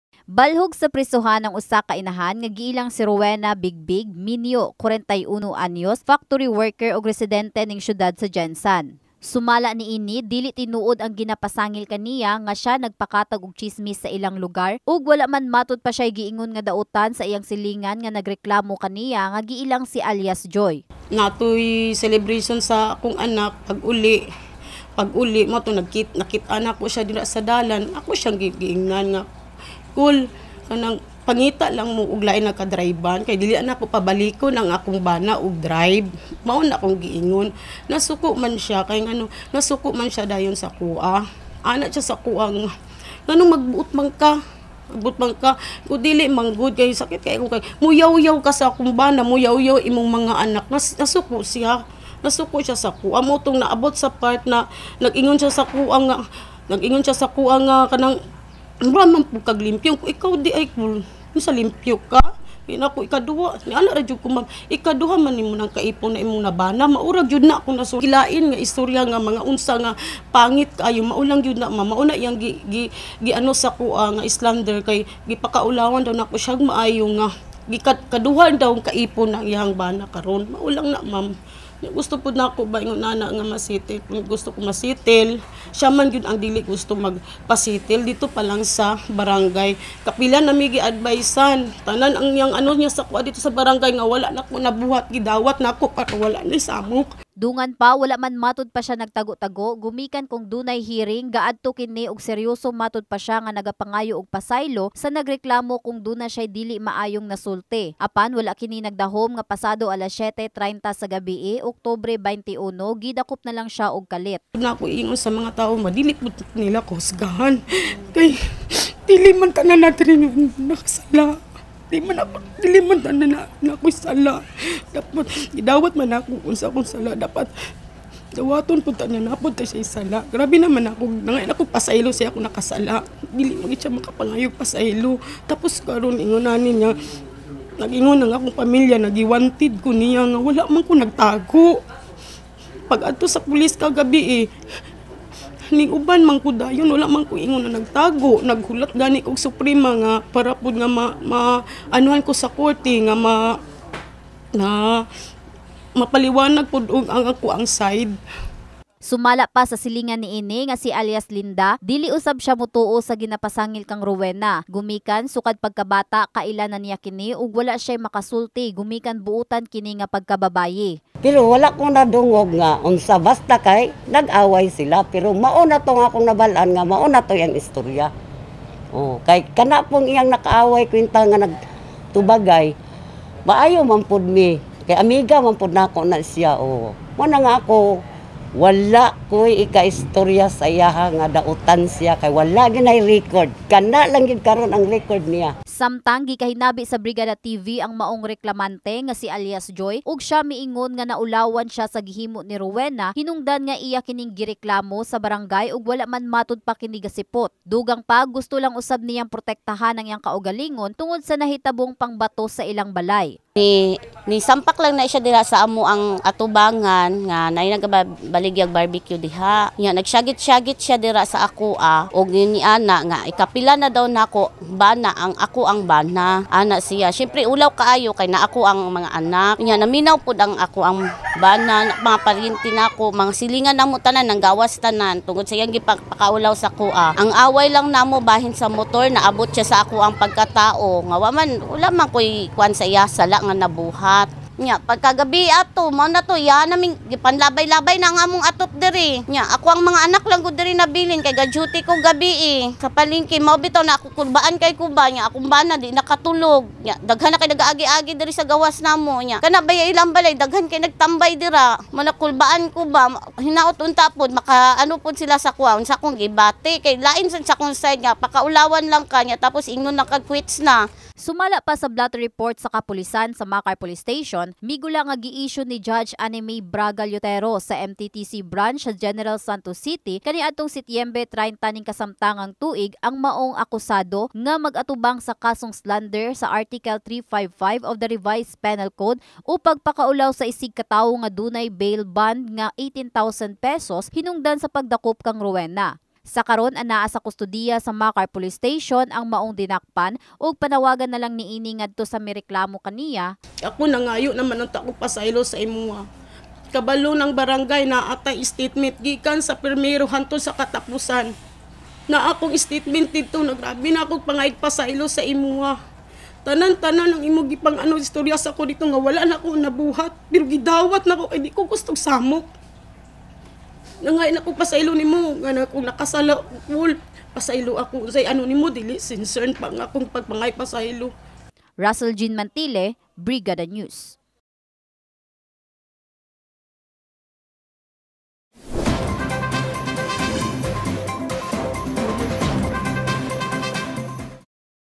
Inahan, sikop sa kasong slander nga gipasaka batok kaniya sa iyang silingan!
GENERAL SANTOS CITY | Inahan, sikop sa kasong slander nga gipasaka batok kaniya sa iyang silingan!